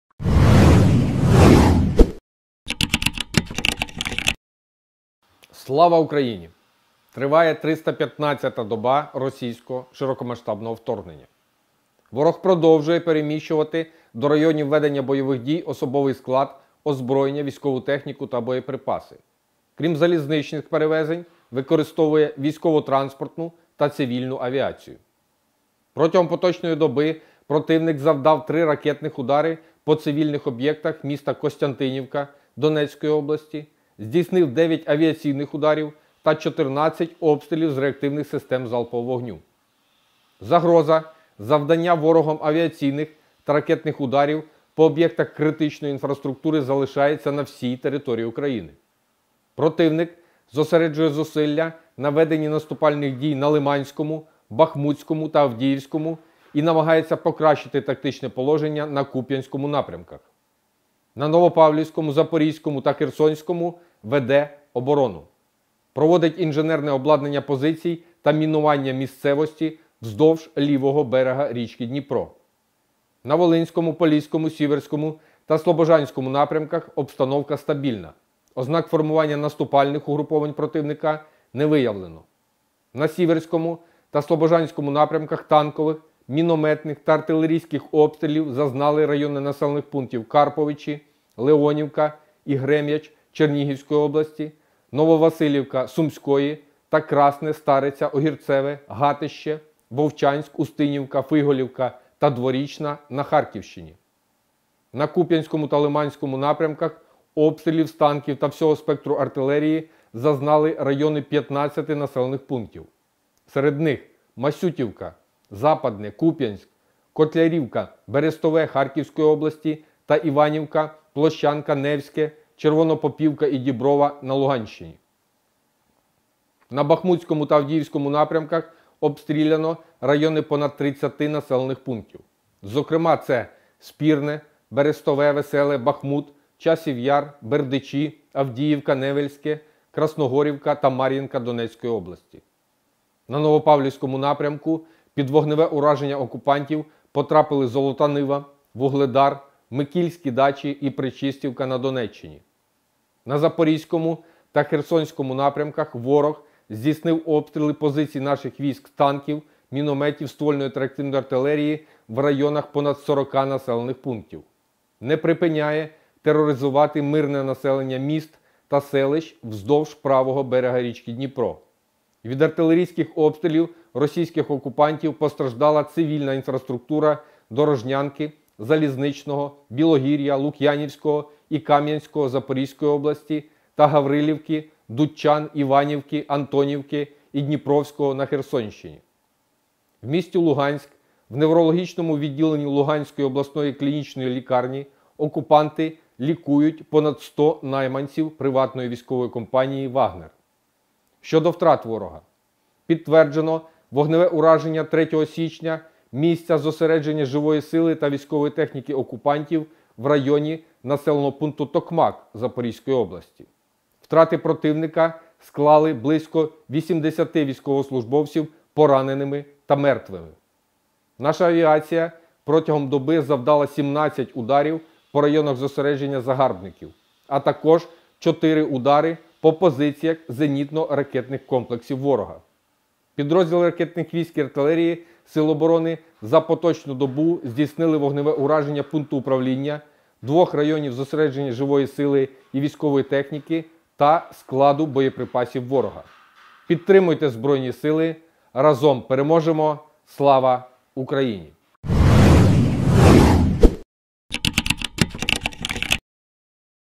Оперативна інформація станом на 18:00 04.01.2023 щодо російського вторгнення
Слава Україні! Триває триста п’ятнадцята доба російського широкомасштабного вторгнення.

Ворог продовжує переміщувати до районів ведення бойових дій особовий склад, озброєння, військову техніку та боєприпаси. Крім залізничних перевезень використовує військово-транспортну та цивільну авіацію.

Протягом поточної доби противник завдав 3 ракетних удари по цивільних об’єктах міста Костянтинівка Донецької області, здійснив 9 авіаційних ударів та 14 обстрілів з реактивних систем залпового вогню.

Загроза завдання ворогом авіаційних та ракетних ударів по об’єктах критичної інфраструктури залишається на всій території України.

Противник зосереджує зусилля на веденні наступальних дій на Лиманському, Бахмутському та Авдіївському і намагається покращити тактичне положення на Куп’янському напрямках. На Новопавлівському, Запорізькому та Херсонському - веде оборону. Проводить інженерне обладнання позицій та мінування місцевості вздовж лівого берега річки Дніпро.

На Волинському, Поліському, Сіверському та Слобожанському напрямках обстановка стабільна, ознак формування наступальних угруповань противника не виявлено.

На Сіверському та Слобожанському напрямках танкових, мінометних та артилерійських обстрілів зазнали райони населених пунктів Карповичі, Леонівка і Грем’яч Чернігівської області; Нововасилівка - Сумської та Красне, Стариця, Огірцеве, Гатище, Вовчанськ, Устинівка, Фиголівка та Дворічна на Харківщині.

На Куп’янському та Лиманському напрямках обстрілів з танків та всього спектру артилерії зазнали райони 15 населених пунктів. Серед них - Масютівка, Западне, Куп’янськ, Котлярівка, Берестове Харківської області та Іванівка, Площанка, Невське, Червонопопівка і Діброва на Луганщині.

На Бахмутському та Авдіївському напрямках обстріляно райони понад 30 населених пунктів. Зокрема це - Спірне, Берестове, Веселе, Бахмут, Часів Яр, Бердичі, Авдіївка, Невельське, Красногорівка та Мар’їнка Донецької області.

На Новопавлівському напрямку під вогневе ураження окупантів потрапили Золота Нива, Вугледар, Микільські Дачі і Пречистівка на Донеччині.

На Запорізькому та Херсонському напрямках ворог здійснив обстріли позицій наших військ з танків, мінометів, ствольної та реактивної артилерії в районах понад 40 населених пунктів. Не припиняє тероризувати мирне населення міст та селищ вздовж правого берега річки Дніпро. Від артилерійських обстрілів російських окупантів постраждала цивільна інфраструктура Дорожнянки, Залізничного, Білогір’я, Лук’янівського і Кам’янського Запорізької області та Гаврилівки, Дудчан, Іванівки, Антонівки і Дніпровського на Херсонщині.

В місті Луганськ, в неврологічному відділені Луганської обласної клінічної лікарні, окупанти лікують понад 100 найманців приватної військової компанії “вагнер”.

Щодо втрат ворога. Підтверджено вогневе ураження 3 січня місця зосередження живої сили та військової техніки окупантів в районі населеного пункту Токмак Запорізької області. Втрати противника склали близько 80 військовослужбовців пораненими та мертвими.

Наша авіація протягом доби завдала 17 ударів по районах зосередження загарбників, а також 4 удари по позиціях зенітно-ракетних комплексів ворога.

Підрозділи ракетних військ і артилерії Сил оборони за поточну добу здійснили вогневе ураження пункту управління, 2 районів зосередження живої сили і військової техніки та складу боєприпасів ворога.

Підтримуйте Збройні Сили! Разом переможемо! Слава Україні!